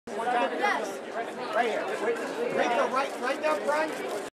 John Gatins at 2013 Writers Guild Awards John Gatins at 2...
John Gatins at 2013 Writers Guild Awards John Gatins at 2013 Writers Guild Awards at J.W. Marriot at L.A. Live on February 17, 2013 in Los Angeles, California